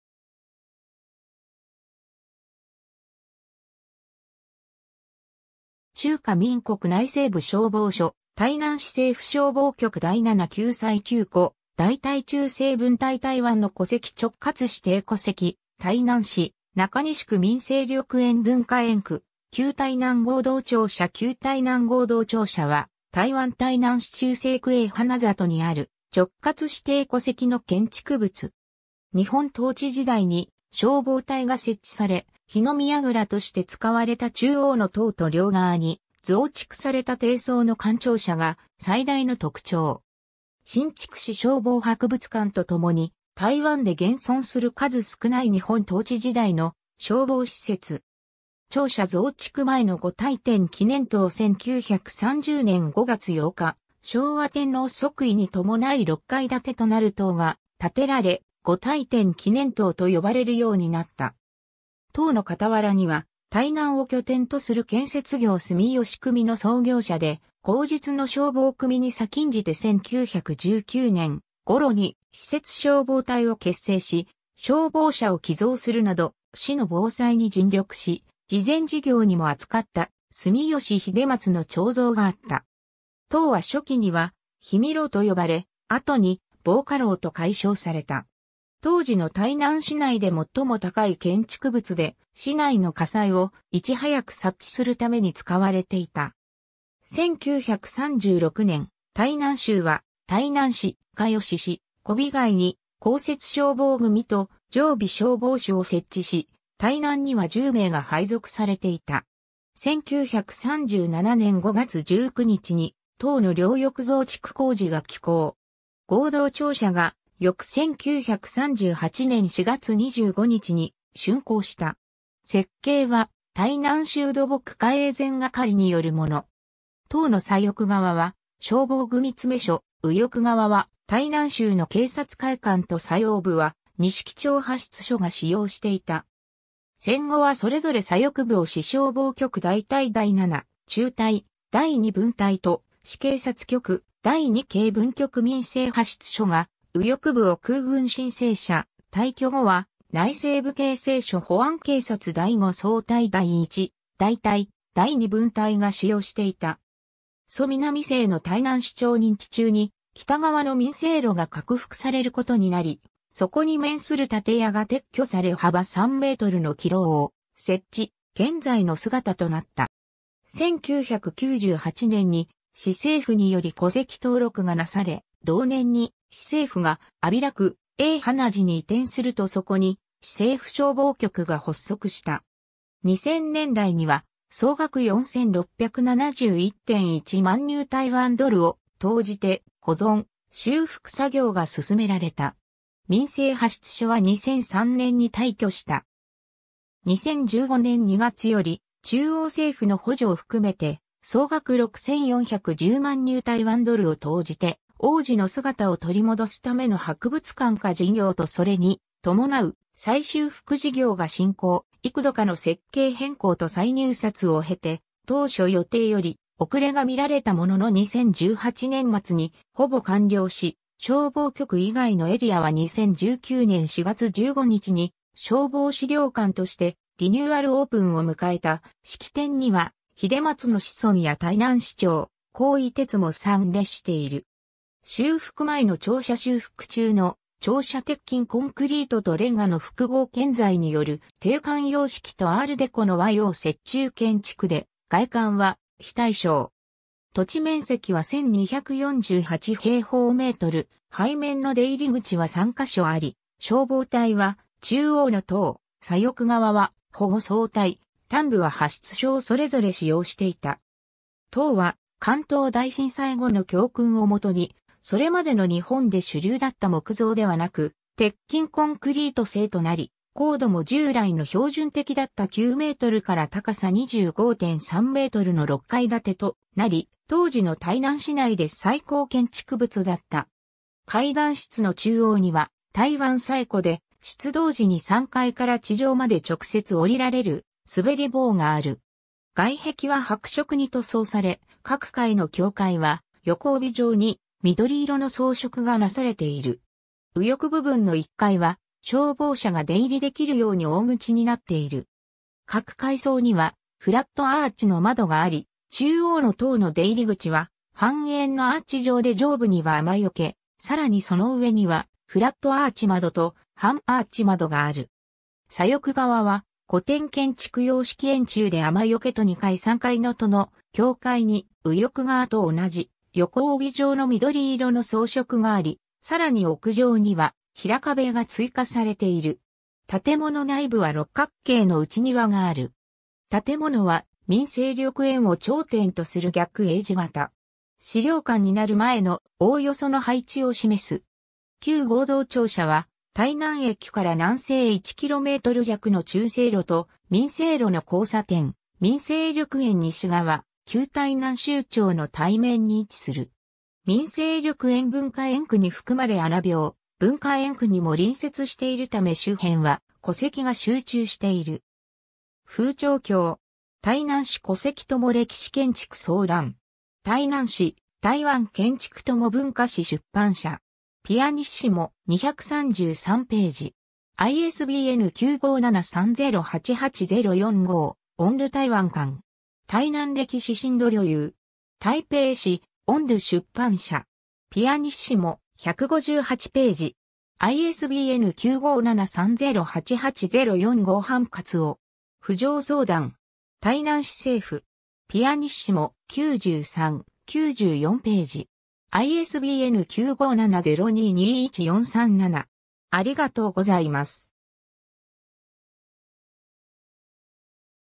旧台南合同庁舎
中西区_(台南市)
中華民国内政部 」 消防署 」 台南市政府消防局 」 第七救災救護大隊中正分隊 台湾の古蹟 」 直轄市定古蹟 」 台南市 」 中西区 」 民生緑園文化園区 」 旧台南合同庁舎 旧台南合同庁舎（きゅうたいなんごうどうちょうしゃ、繁体字中国語: 原臺南合同廳舍）は台湾台南市中西区永華里にある直轄市定古蹟の建築物。
 日本統治時代に消防隊が設置され、火の見櫓として使われた中央の塔と両側に増築された低層の官庁舎が最大の特徴。
 新竹市消防博物館とともに台湾で現存する数少ない日本統治時代の消防施設。
 庁舎増築前の御大典紀念塔（1930年） 1930年（昭和5年）5月8日、昭和天皇即位に伴い6階建てとなる塔が建てられ、「御大典紀念塔」と呼ばれるようになった。
 塔の傍には台南を拠点とする建設業住吉組の創業者で、後述の消防組に先んじて1919年（大正8年）ごろに私設消防隊を結成し、消防車を寄贈するなど市の防災に尽力し、慈善事業にも篤かった住吉秀松（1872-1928）の彫像があった。
塔は初期には「火見楼」と呼ばれ、後に「望火楼」と改称された。
当時の台南市内で最も高い建築物で、市内の火災をいち早く察知するために使われていた。
 1936年（昭和11年）、台南州は台南市、嘉義市、虎尾街に『公設消防組』と『常備消防手』を設置し、台南には10名が配属されていた。
 1937年（昭和12年）5月19日に塔の両翼増築工事が起工。
合同庁舎が翌1938年4月25日に竣工した。
設計は台南州土木課営繕係によるもの。
 塔の左翼側は消防組詰所、右翼側は台南州の警察会館と最奥部は錦町派出所が使用していた。
 戦後はそれぞれ左翼部を市消防局大隊第七中隊第二分隊と市警察局第二警分局民生派出所が、右翼部を空軍新生社、退去後は内政部警政署 保安警察第五総隊（保五総隊）第一大隊第二分隊が使用していた。
蘇南成の台南市長任期中（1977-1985年）に北側の民生路が拡幅されることになり、そこに面する建屋が撤去され幅3メートルの騎楼を設置、現在の姿となった。
 1998年に市政府により古蹟登録がなされ、同年に市政府が安平区永華路に移転するとそこに市政府消防局が発足した。
（戦後、市の消防組織が所属する母体は市警察局、台湾省政府、内政部などと変遷している。） 2000年代には総額4,671.1万ニュー台湾ドルを投じて保存・修復作業が進められた。
 民生派出所は2003年に退去した。
 2015年2月より中央政府の補助を含めて総額6,410万ニュー台湾ドルを投じて往時の姿を取り戻すための博物館化事業とそれに伴う再修復事業が進行、幾度かの設計変更と再入札を経て、当初予定より遅れがみられたものの2018年末にほぼ完了し、消防局以外のエリアは2019年4月15日に消防史料館としてリニューアルオープンを迎えた、式典には秀松の子孫や台南市長黄偉哲も参列している。
 修復前の庁舎（2012年） 修復中の庁舎（2016年） 鉄筋コンクリートと煉瓦の複合建材による帝冠様式とアール・デコの和洋折衷建築で、外観は非対称。
土地面積は1,248平方メートル、 背面の出入り口は3か所あり、消防隊は中央の塔、左翼側は保五総隊、端部は派出所をそれぞれ使用していた。
 塔は関東大震災後の教訓を元にそれまでの日本で主流だった木造ではなく鉄筋コンクリート製となり、高度も従来の標準的だった9メートルから高さ25.3メートルの6階建てとなり、当時の台南市内で最高建築物だった。
 階段室の中央には台湾最古で、出動時に3階から地上まで直接降りられる滑り棒（en:Fireman's pole）がある。
 外壁は白色に塗装され、各階の境界は横帯状に緑色の装飾がなされている。
右翼部分の1階は消防車が出入りできるように大口になっている。
各階層にはフラットアーチの窓があり、中央の塔の出入口は半円のアーチ状で上部には雨除け、さらにその上にはフラットアーチ窓と半アーチ窓がある。
 左翼側は古典建築様式円柱で雨除けと2階・3階のとの境界に右翼側と同じ横帯状の緑色の装飾があり、さらに屋上には平壁が追加されている。
建物内部は六角形の内庭がある。
 建物は民生緑園を頂点とする逆A字型。
史料館になる前のおおよその配置を示す。
 旧合同庁舎は台南駅から南西へ1km弱の中正路と民生路の交差点『民生緑園』（湯徳章紀念公園、大正公園）西側、...